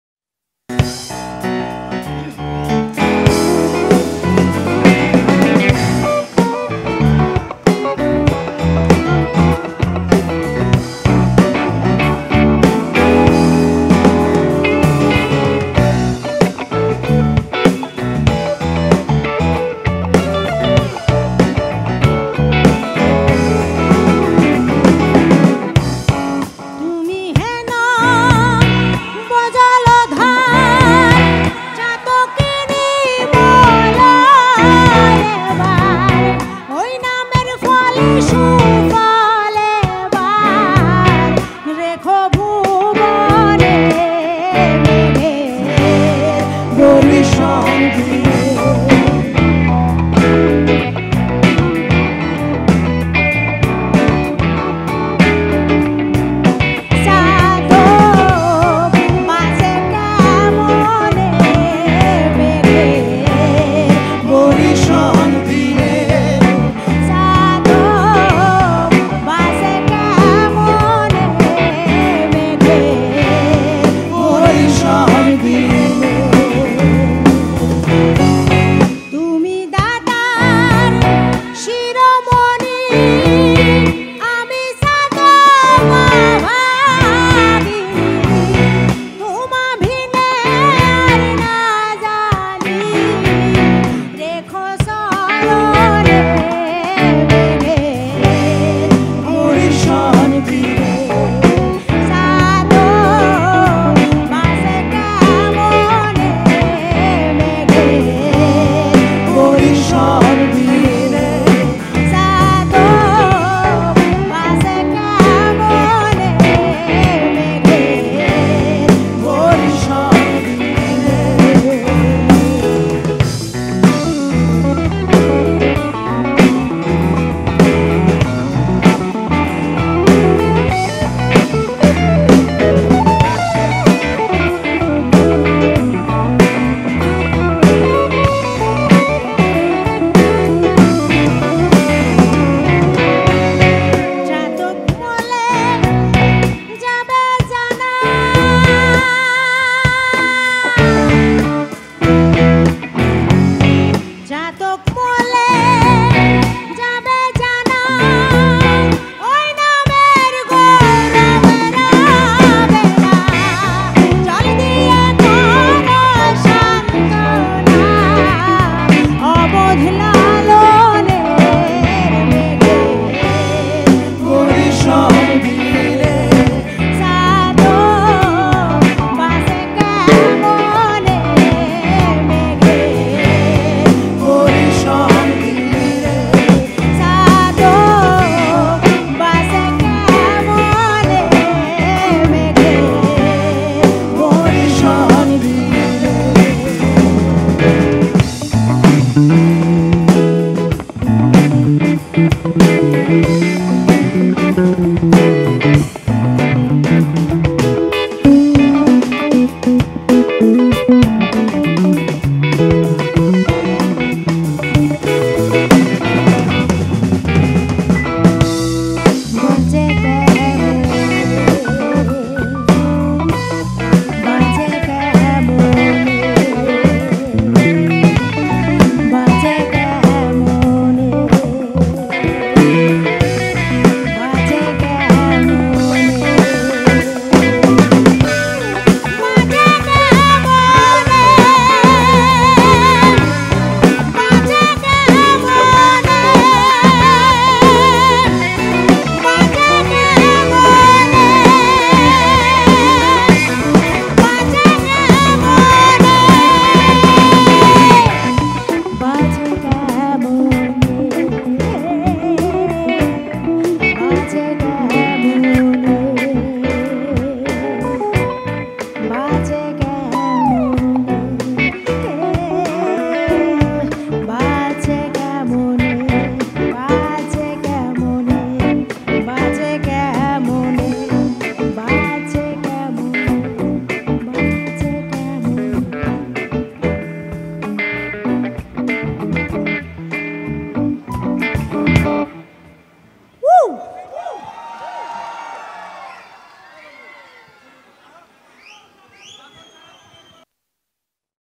সাইজীর দোল উৎসব ও বঙ্গবন্ধুর জন্মদিনে ব্যান্ড সুমির অস্তির লালনগীতি গান | Band Sumi | Lalon song
Welcome all !